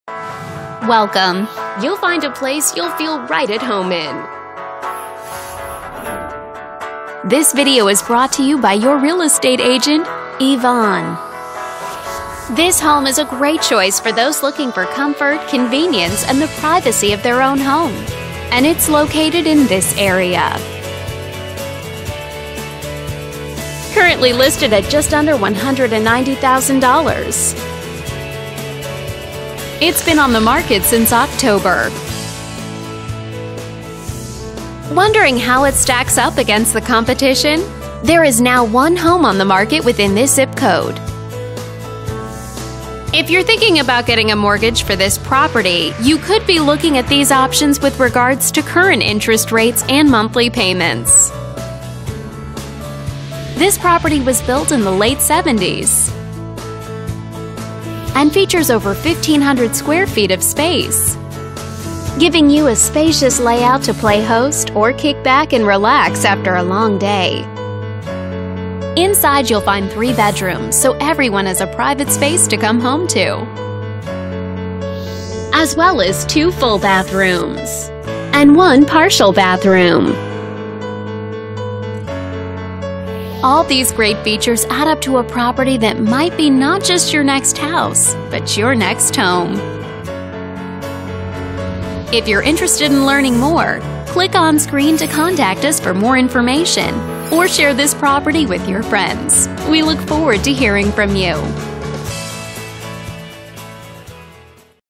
Homes for Sale in Craig 1085 Kowach Drive
Make this your home!  Greet guests into the formal living room and large open kitchen area.  Master and two additional bedrooms plus two full baths privately located on the upper lever.  Entertain game night in the cozy family room downstairs.  Nice attached two car garage.  Fenced back yard.  Mature landscape in the front.  Easy to show!